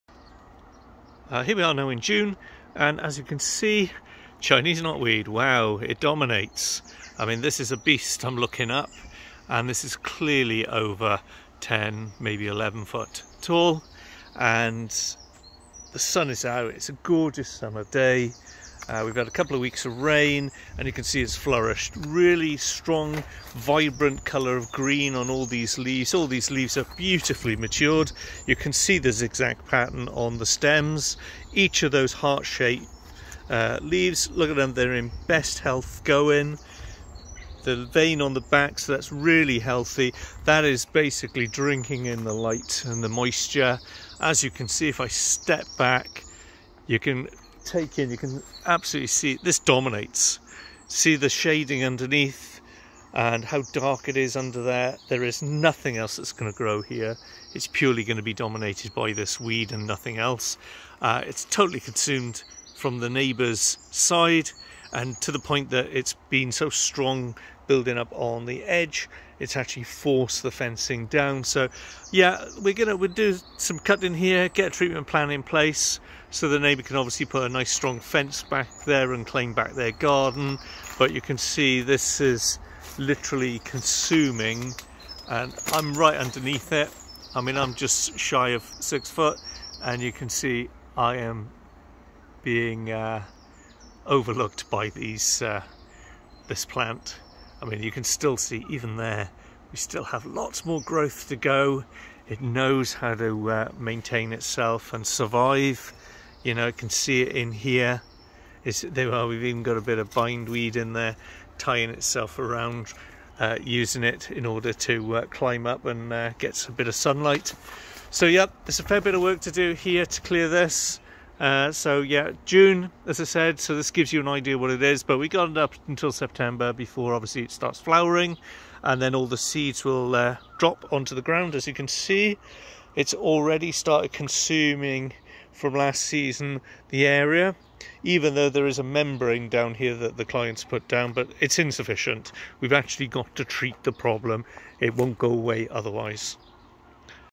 Mix Of Chinese Knotweed And Bindweed In A Garden 🏡 Invasive Weeds
Chinese knotweed and bindweed can quickly take over a garden, causing damage to both the plants and the soil.

To identify Chinese knotweed, look for its tall, bamboo-like stems with distinctive knot-like joints and broad, flat leaves. The plant produces small white flowers in dense clusters and spreads rapidly via underground roots.

Bindweed, on the other hand, is a trailing vine with spiralling stems and trumpet-shaped white or pink flowers. Its leaves are arrowhead-shaped and grow alternately along the stem.

If you suspect you have a problem with Chinese knotweed or bindweed in your garden, it's important to take action to prevent it from spreading and causing further harm. One option is to manually remove the plants, being sure to get all the roots to prevent regrowth. Another option is to use herbicides, but be sure to follow all label instructions and take precautions to avoid harm to non-target species.

By learning about these invasive weeds and taking appropriate action, we can help protect our gardens and the surrounding environment from their harmful effects.